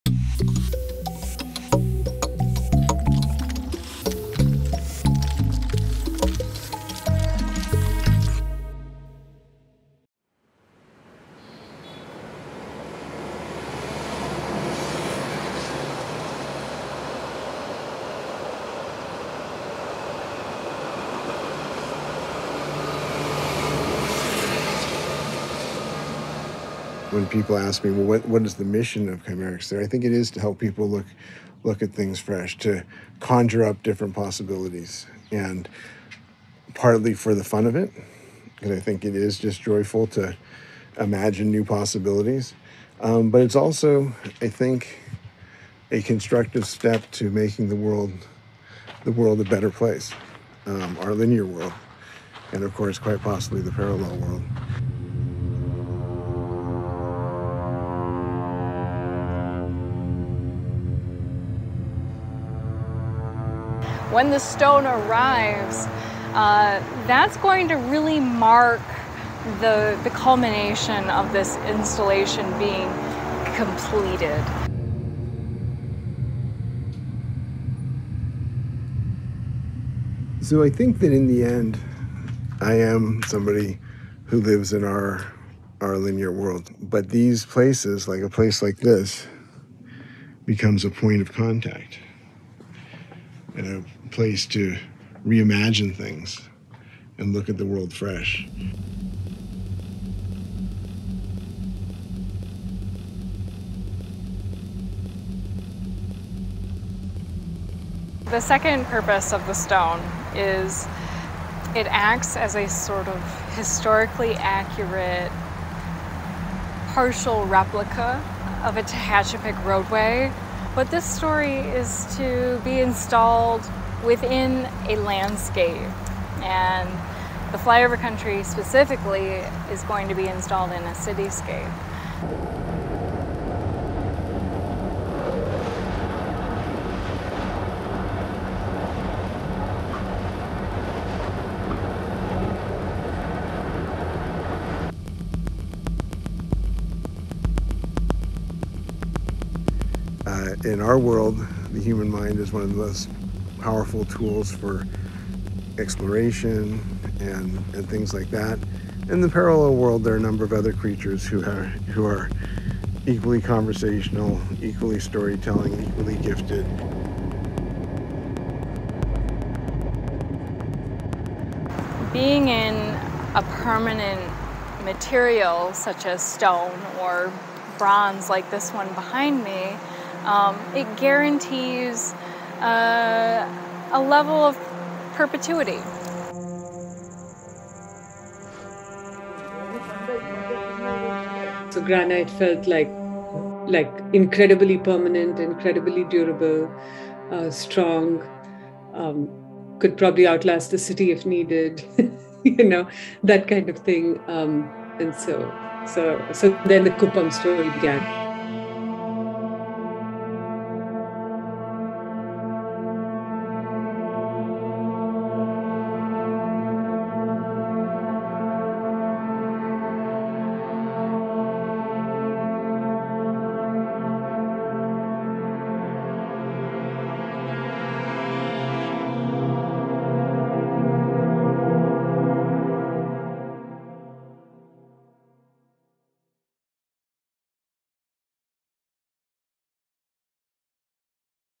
Ep 02: Kcymaerxthaere Flyover Country
Geographer-at-Large Eames Demetrios, Kcymaerxthaere team Project Manager Amelia Volwiler Stanley, and Jaaga / BeFantastic founder Archana Prasad talk about the journeys the parallel story must take — from imagination to physical stone, and how it brings the team to Kuppam for an adventure in stone-carving.